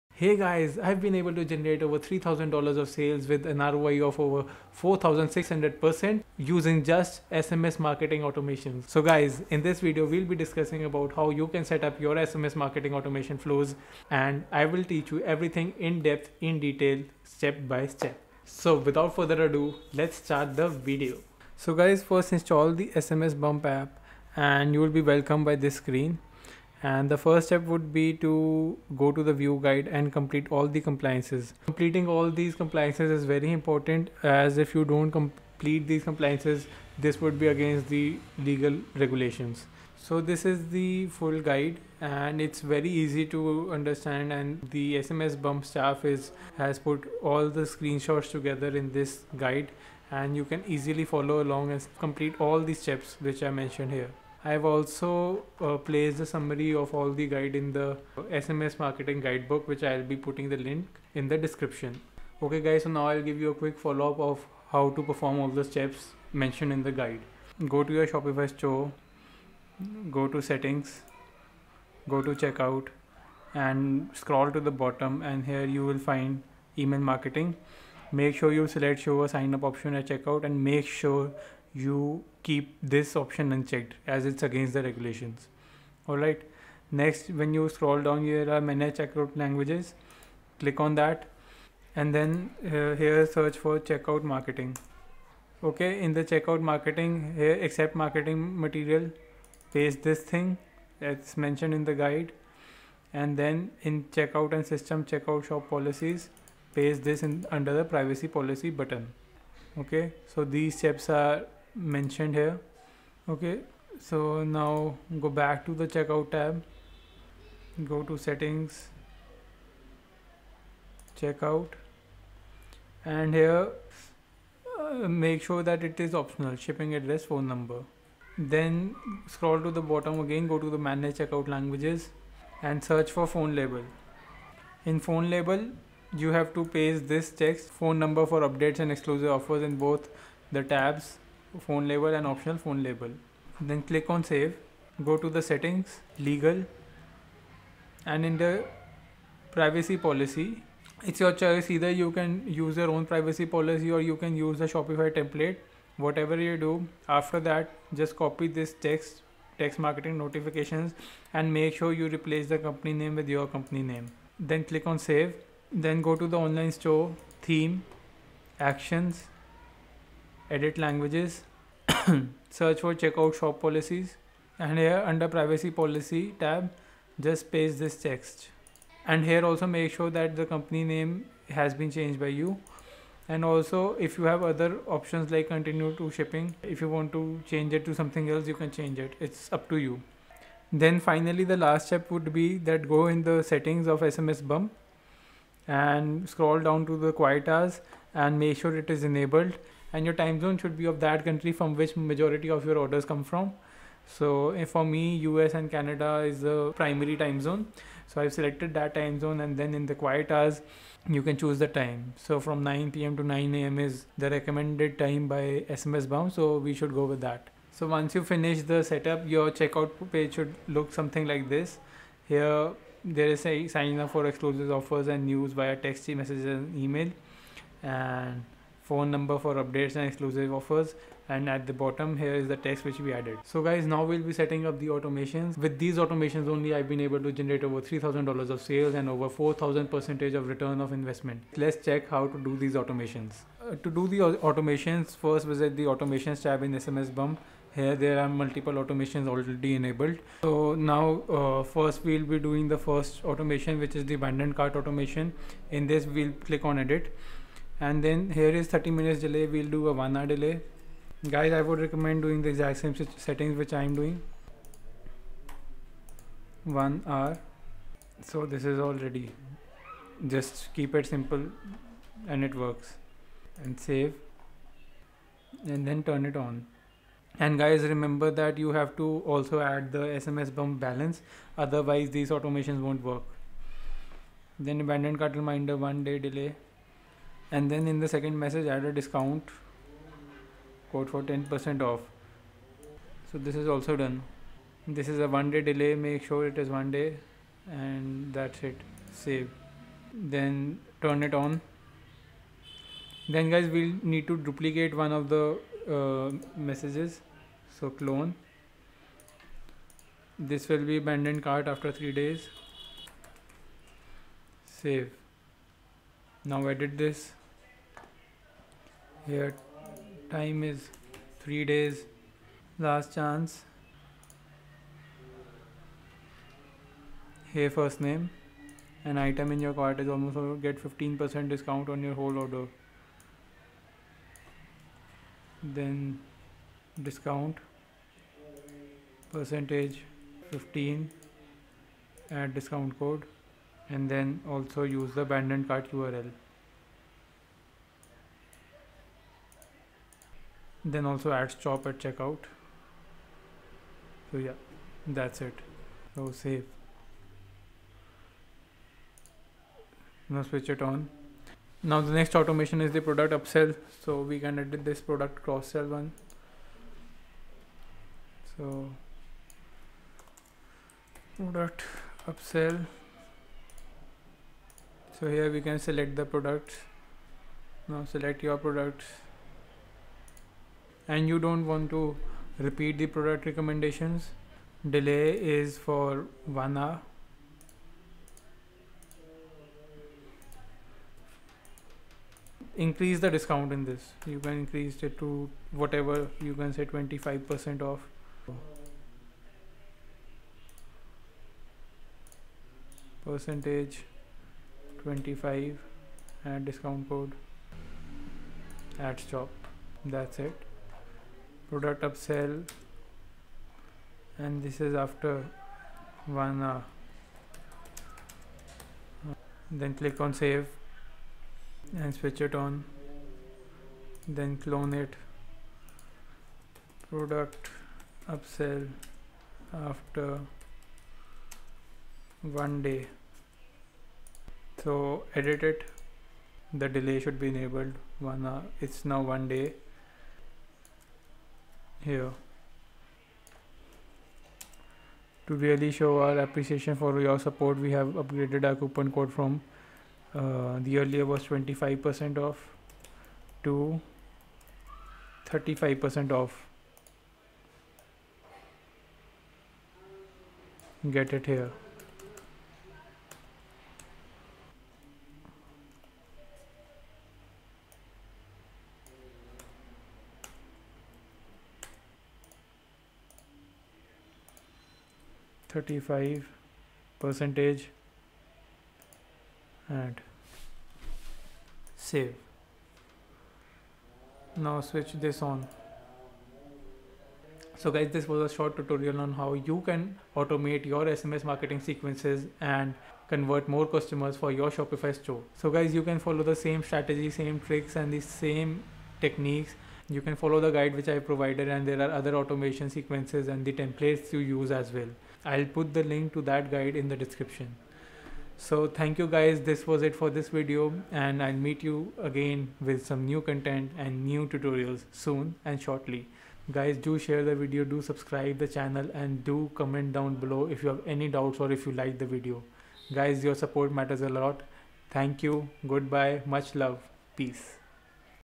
SMS MARKETING SETUP FOR BEGINNERS - SHOPIFY SMS BUMP TUTORIAL | SMS AUTOMATION FLOWS UNLOCKED
The Beginner's Guide to SMS Marketing in 2020 | SMS marketing is still one of the cheapest and most effective ways to get targeted visitors hitting your website on a regular basis and they actually CONVERT. Today, I'm going to break down the beginners guide to SMS marketing in 2020.

RESOURCES & LINKS:
SETUP SECTION

1.     Go to View Guide in Welcome Section of Dashboard and set up your compliance.

2.     Enable Marketing Checkbox and ensure it is not preselected as this is against regulations.

3.     Go back to your Store - Settings - Checkout - Email marketing and make sure the preselect option is disabled.

4.     Scroll down to Checkout Language and select Manage Check Out.

5.     Go back to SMS Bump and copy (CMD + C / Control +C) the language provided below:
a.     ( Sign up for exclusive offers and news via text messages and/or email )

6.     Go back to store and select Settings - Checkout - Check out language and select manage check out language.

7.     Click Control F to search for phrase and type in Accept Marketing/ Checkbox Label at top of page in search bar.

8.     Paste the text from line 5 into the Accept marketing Checkbox section and Save Settings.

9.     Go to your cart and it should look like below.

10.  Go to Shipping address phone number and make it Optional or Required.

11.  Go back to Shopify Store, select Form Options - Shipping Address Phone Numbers and select optional or required and save your settings.

12.  Go back to SMSBump Instructions. Copy and Paste text on store

13.  Go back to store - Manage Languages and search at top for Phone label.

14.  Paste the information provided by SMSBump into the field.
a.      ( Phone number and email for exclusive offers. )

15.  Your page should look like below. This is where you customers will share their phone number for texts.

16.  Next go to Privacy Policy. You can go back to your SMSBump instructions and copy and paste the necessary language to put on your store.

18.  Paste provided text into the field as instructed and Save Settings.
Consent is not a condition of purchase. Reply STOP to unsubscribe. HELP for help. Msg & Data rates may apply.

19.  Check out Footer Language

20.  Go to theme settings - actions and select Edit Languages.

21.  Select Checkout and Systems and search for phrase Checkout Shop Policy.

22.  Paste the data in the field and Save Settings
a.     (I consent to receive recurring automated marketing by text message through an automated text dialing system. Consent is not a condition to purchase. STOP to cancel, HELP for help. Message & Data rates may apply. View Privacy Policy.)

23.  Enable Quiet Hours. Important!

24.  Set up Complete. Enjoy!

AUTOMATIONS SETUP

Abandon cart recovery sequence:
SMS Text 1 (send after 1 hour)

Product Upsell Sequence:
SMS Text 1 (send after 1 hour) (SiteName): Hello, (BillingFirstName). Thanks for purchasing (BoughtProductName). As a huge thank you for your support we like to offer you a limited time 4O% OFF from (RecommendProductName).

SMS Text 2 (send after 1 day) (SiteName): Hello, (BillingFirstName).

Customer winback sequence:
SMS Text 1 (SiteName): Hi (FirstName) we noticed it has been a while since your last order.

SMS Text 2 (send after 3 days) (SiteName): Hi (FirstName) we noticed you didn't order anything from our new product range let me tempt you with a 40% OFF on your next purchase (SiteUrl)

Shipping confirmation flow:
Wait 12 hours
Add action step: (SiteName):  If your tracking number isn't working please allow up to 3 working days for the tracking number to update.